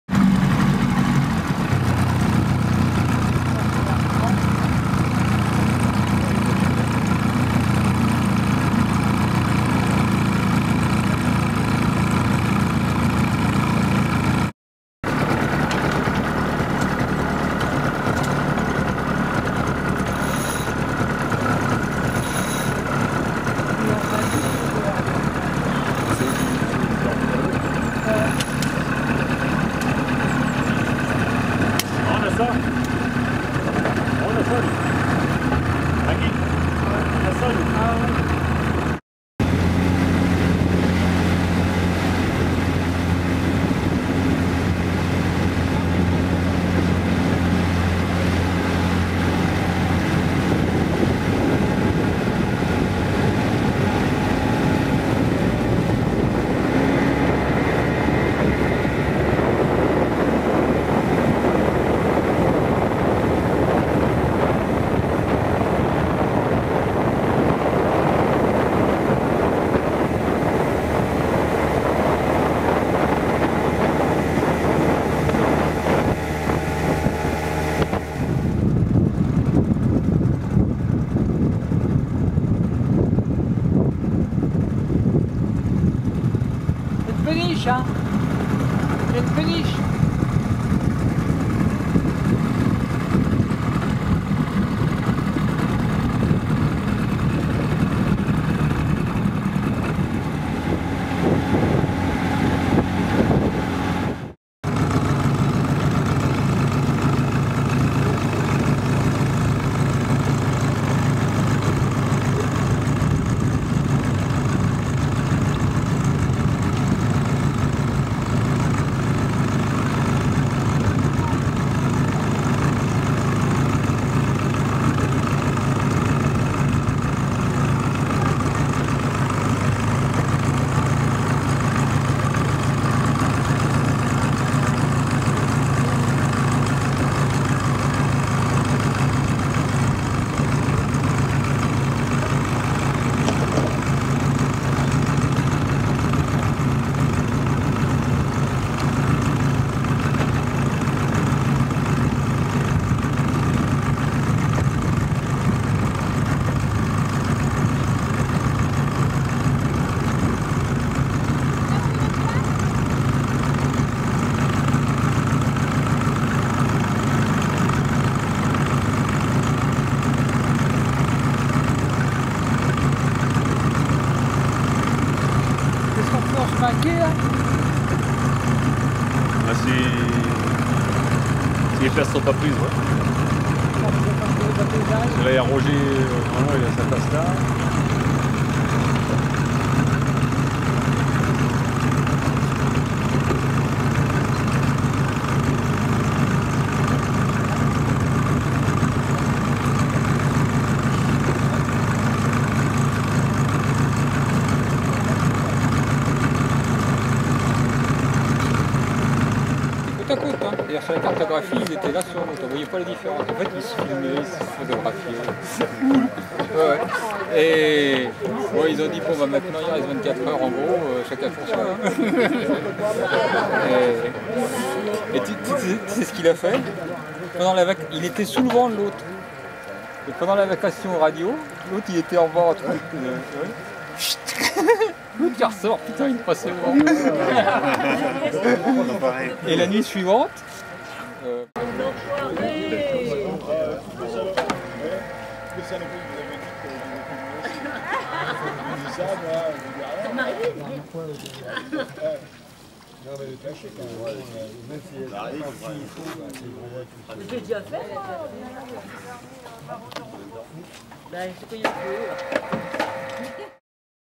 6H de Vaux
Quelques images d'une régate à Vaux sur Seine : les 6 heures de Vaux